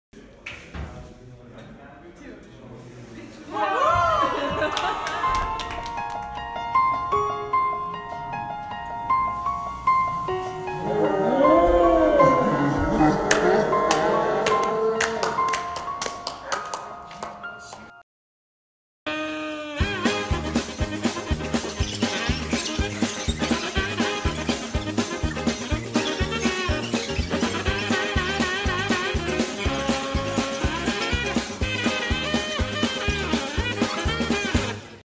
Paper Pick-up Challenge: GoodCall Prague offices
Podaří se vám zvednout přeložený papír A4 ústy při postoji na jedné noze a bez pomoci rukou jako naší Natálii? :) I kdyby ne - garantujeme, že si u nezdařených pokusů užijete hodně legrace.